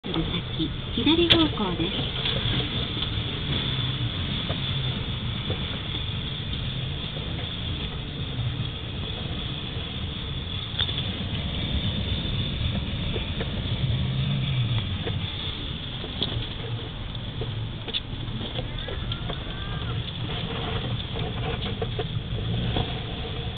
P2071185.AVI
豪雪の桐の木平キャンプ場から、ワイドハイエースで決死の下山。通った後は、大きなU字溝の出来上がり！